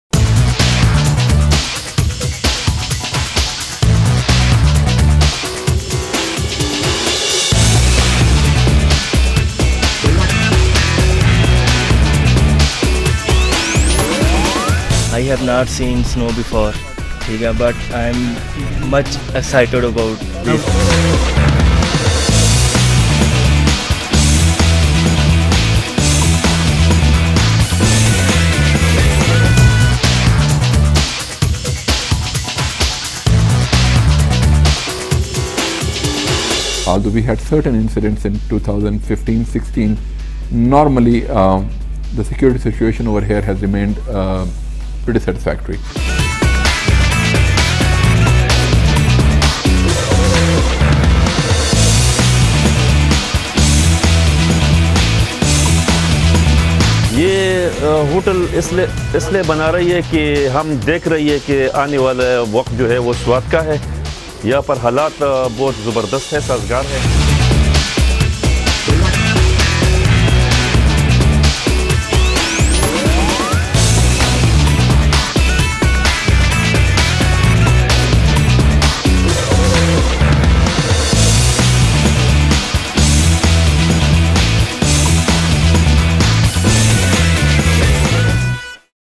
„Реинкарниран“ ски центар во Пакистан
Ски центарот, во провинцијата Сват, во Пакистан, пред 10 години беше уништен од Талибанците кои остро се противат на скијањето како „неисламски“ спорт. Меѓутоа, ски центарот сега повторно е безбеден, привлекува домашни и странски инвеститори и локални љубители на зимските спортови.